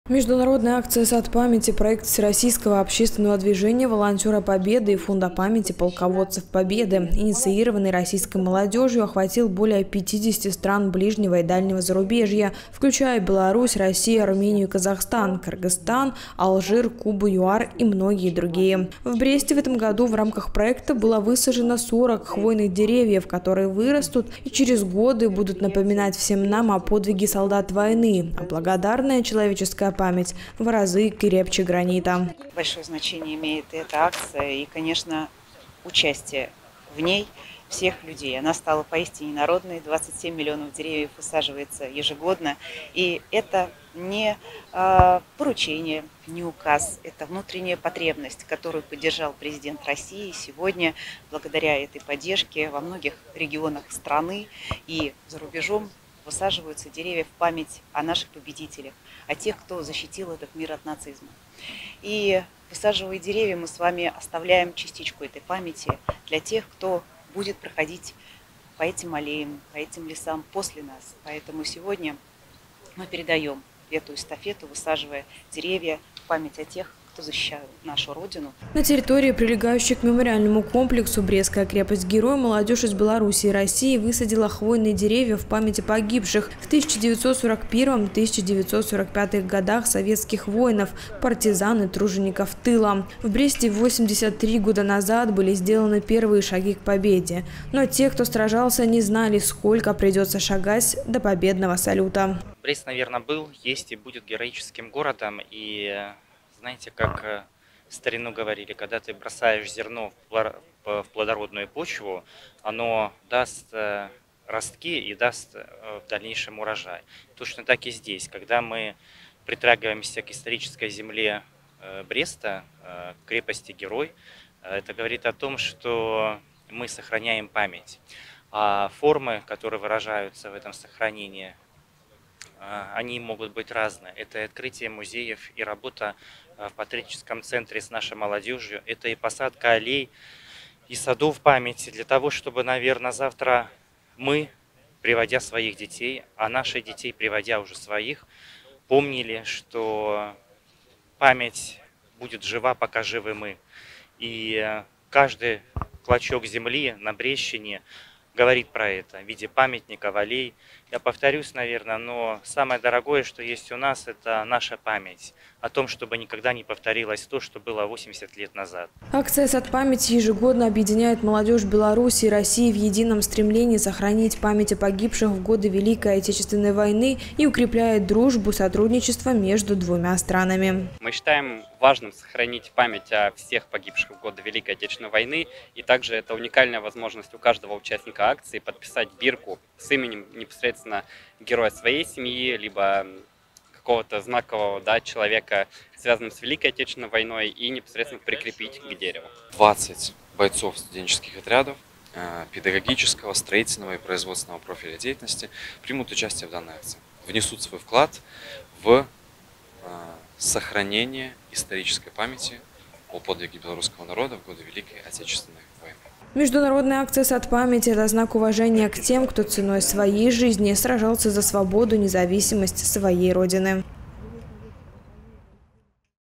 2024-06-25 Международная акция «Сад памяти»
В городе Бресте, в День памяти и скорби прошла международная акция «Сад памяти». Пятый год подряд – в России, Беларуси и за рубежом высаживают 27 миллионов деревьев в память о тех, кто сражался за наше будущее в годы Великой Отечественной войны. В этом году Республика Беларусь празднует 80-летие освобождения от немецко-фашистских захватчиков. В честь этого события в Бресте руководством города, депутатом Государственной Думы Федерального собрания Российской Федерации и заместителем Госдумы Анной Кузнецовой, а также молодёжью двух государств была заложена памятная аллея из 40 деревьев. Символично, что зеленые памятники появились в Бресте, именно там, где 83 года началась Великая Отечественная война.

*Наши контакты:*
✅Телефон горячей линии:
54-63-67